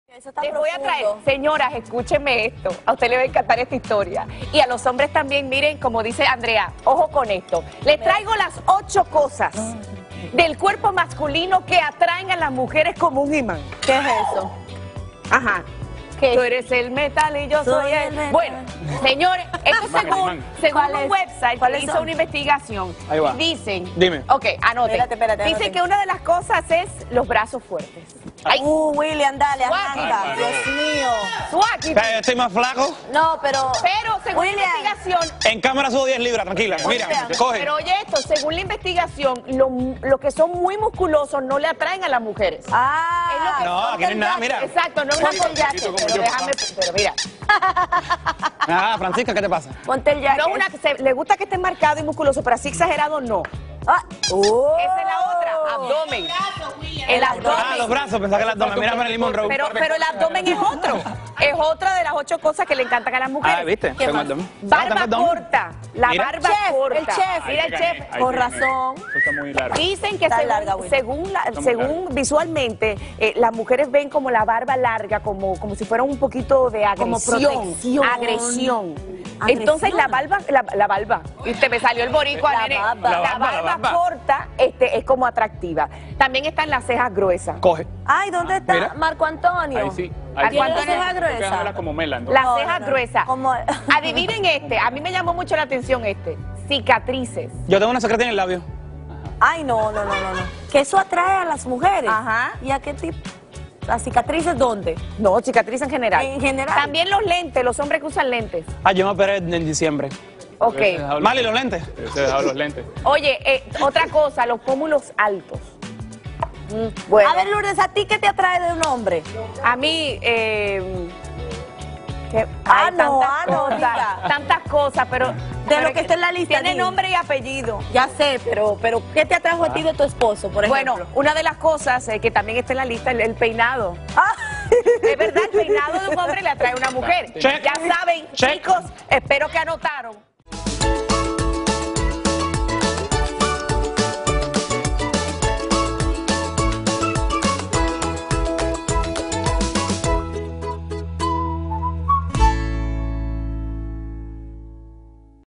William tiene un cuerpo que fascina a las mujeres
¿Te gustan los hombres musculosos, con barba, y abdomen marcado? Francisca y Lourdes descubrieron que William y el Chef Jesús cumplen con varios de esos requisitos.